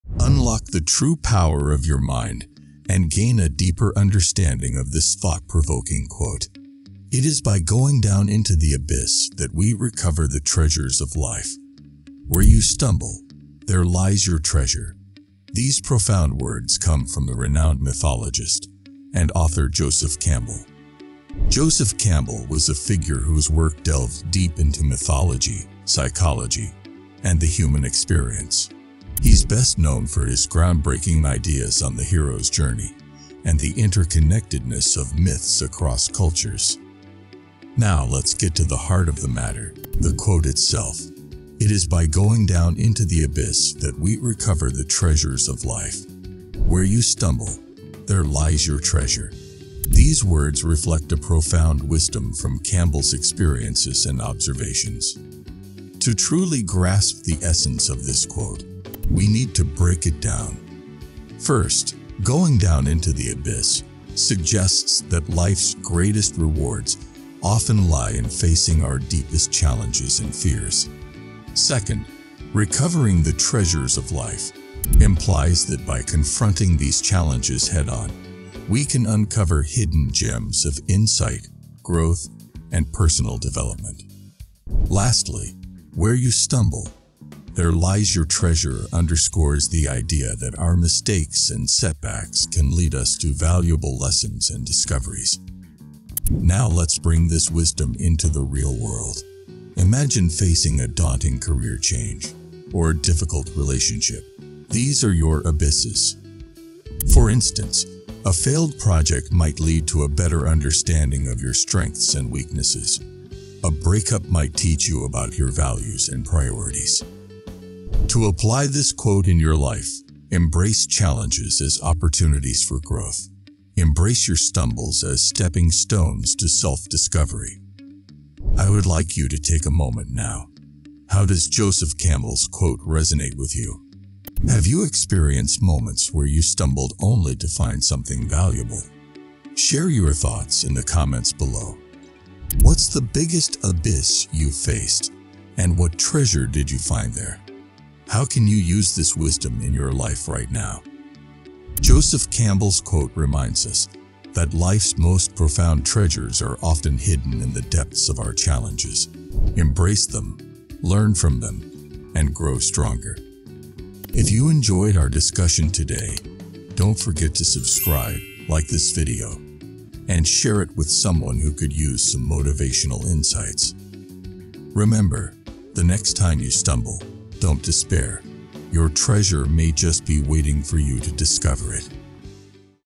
Wisdom for Turning Stumbles into Triumphs
In the depths of the human heart, where life's profound mysteries reside, lies an abyss, a challenging and often daunting terrain. We often stumble within this abyss, but fear not, for here, amidst the challenges and stumbles, lie the hidden treasures waiting to be uncovered. Welcome to our motivational journey of self-discovery, where we explore the wisdom and inspiration that can help you foster a successful mindset and undergo a transformative life journey.

In this engaging video, we examine the powerful quote, "It is by going down into the abyss that we recover the treasures of life. Where you stumble, there lies your treasure." These words resonate deeply with those seeking motivation, personal growth, and cultivating a positive mindset for success.

Life's struggles and stumbling blocks are inevitable, but what if we told you that they are not obstacles but stepping stones? By embracing these challenges, we embark on a journey of self-discovery, unearthing the hidden gems within ourselves and our lives. It's a path less traveled but one that promises profound transformation.

Join us as we explore the essence of discovering life's treasures within the context of an inspirational quote for success. We will share real-life stories and practical insights that illustrate the profound wisdom behind these words. Whether you face career dilemmas, relationship hurdles, or personal setbacks, this video offers a guiding light to help you navigate your unique life journey with a positive mindset for success.

Don't miss the opportunity to unlock your inner hero and learn how to turn stumbling blocks into valuable life experiences. As we discuss Joseph Campbell's wisdom, you will find inspiration and motivation to confront your abysses, eager to recover the awaited treasures.

Remember, life's challenges are not roadblocks but catalysts for growth and transformation. By embracing the abyss, you're taking the first step towards discovering life's treasures and nurturing a positive mindset that will pave the way for success in all aspects of your life.

Join the conversation by sharing your own stories of self-discovery and personal growth in the comments below. Let's embark on this transformative journey together, and together, we will uncover the hidden gems within our own lives.

Want to spread the word about this awesome video? You won't want to miss out!